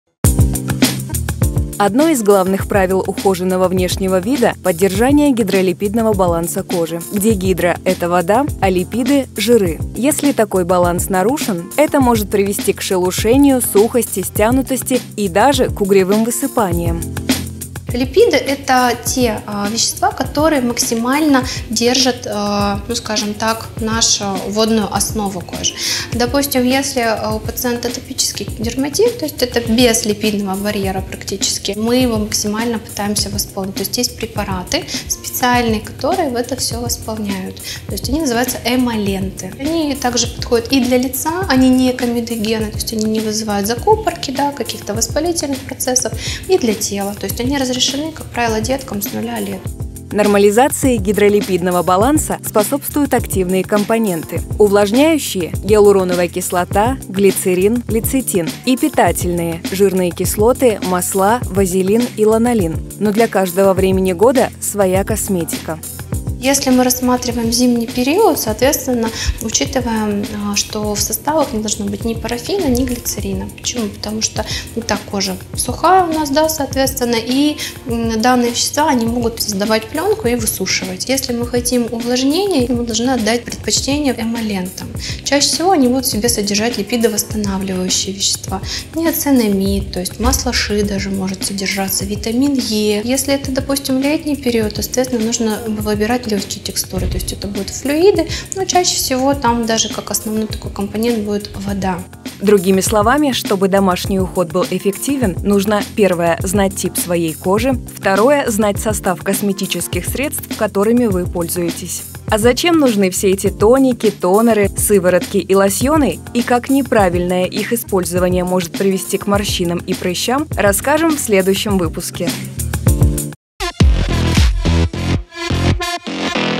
ПОЛЕЗНЫЕ СОВЕТЫ КОСМЕТОЛОГА: как ухаживать за кожей и избавиться от проблем / правила ухода за кожей
Что такое гидролипидный баланс кожи и зачем его поддерживать? Как ухаживать за кожей зимой и летом? Здесь полезные советы по уходу от врача-дерматовенеролга и косметолога!